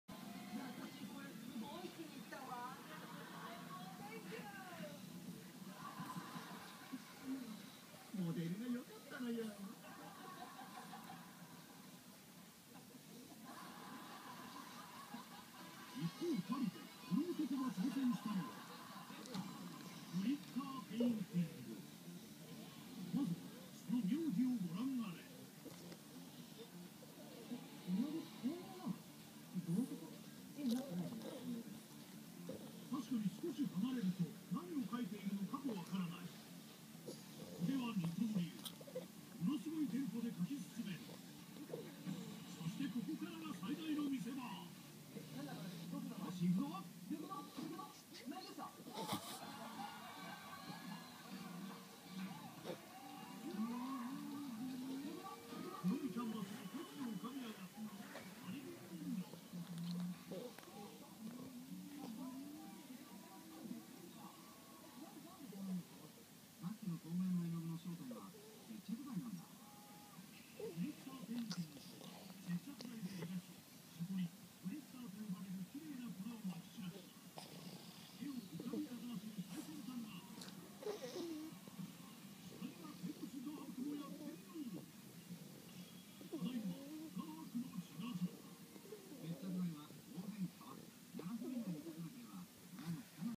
ベッドで気持ちよさそうに寝るポロロ♪ French bulldog PORORO 072 フレンチブルドッグ ポロロ
French bulldog PORORO 072 フレンチブルドッグ ポロロ ベッドで気持ちよさそうに寝るポロロ♪